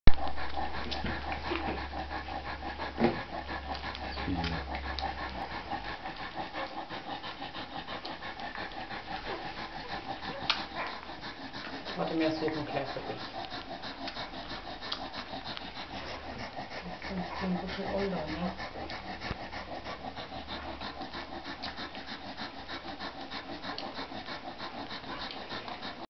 Welpen nach 1 Woche
Hier seht Ihr die 7 Tage alten Welpen an Maddy angelegt.